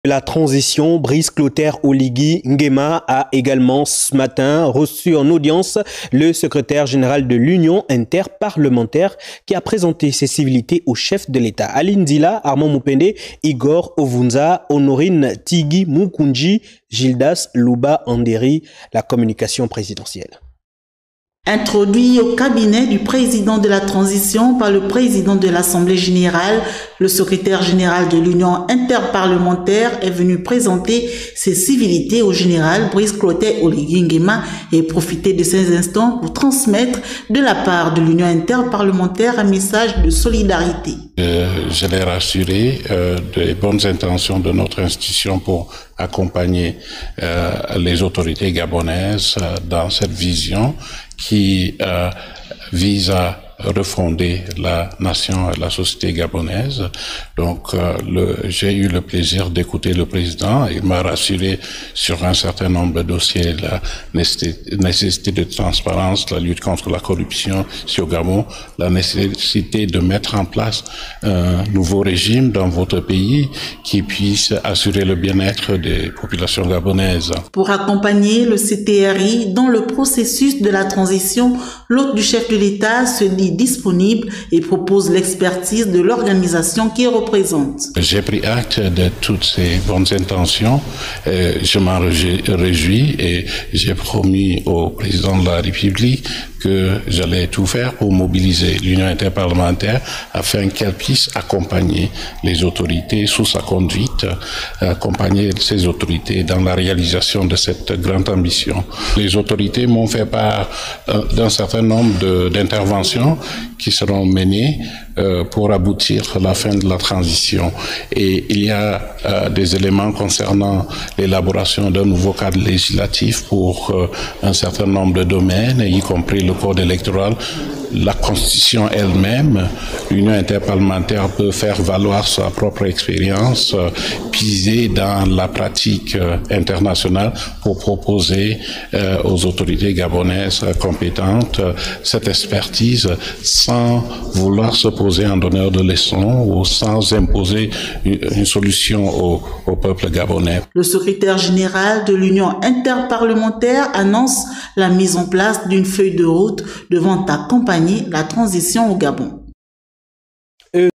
Présidence : le Secrétairegénéral de l'U.I.P Martin Chungong reçu par le Pdt Oligui Nguema
Bonjour et bienvenue sur ma chaîne dans cet extrait, le président de la transition chef de l'Etat Oligui Nguema, a reçu en audience le secrétaire général de l'union Inter parlementaire monsieur Martin Chungong.
L'union inter parlementaire souhaite accompagner le Gabon et le CTRI dans la restauration des institutions, sans pour autant imposer un veto. Leur accompagnement serait sans conditions et tant mieux.